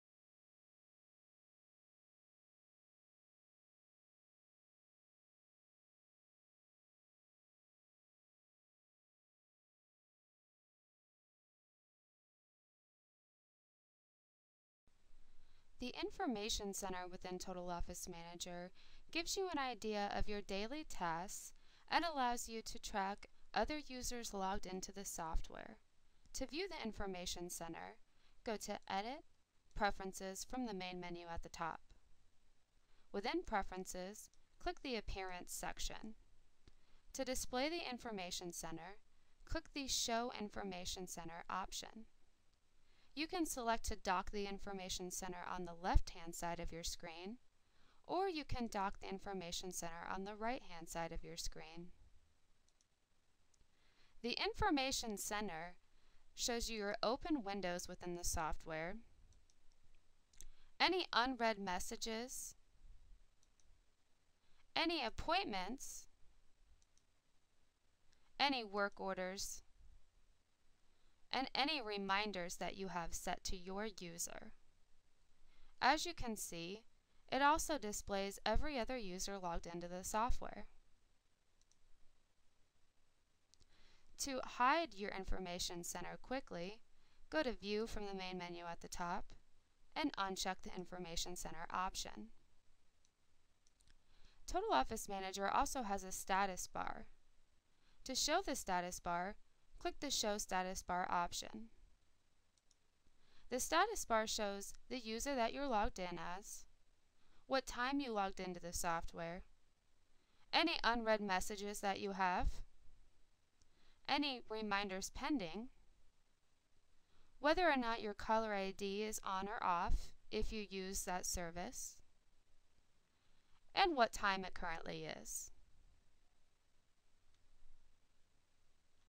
Info Center and Status Bar
Using the Information Center and Status Bar in Total Office Manager.